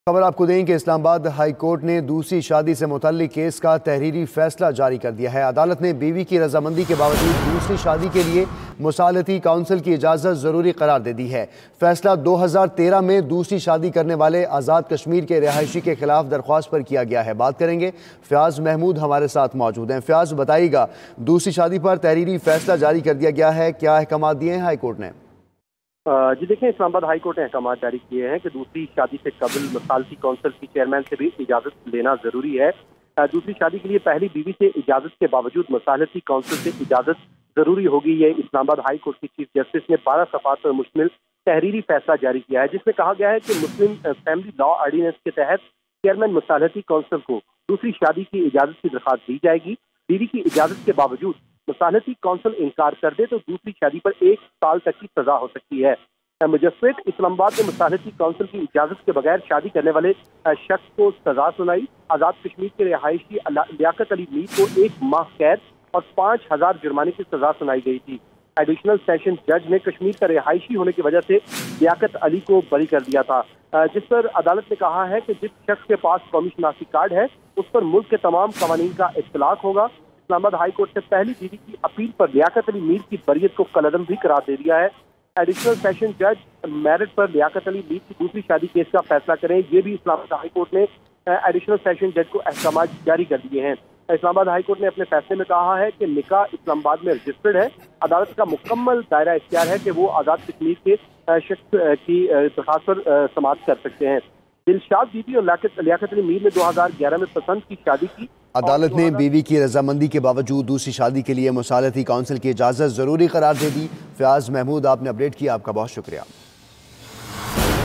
IHC makes second marriage conditional to arbitration council’s approval | 24 June 2019 | 92NewsHD
Watch: IHC makes second marriage conditional to arbitration council’s approval | 24 June 2019 | 92NewsHD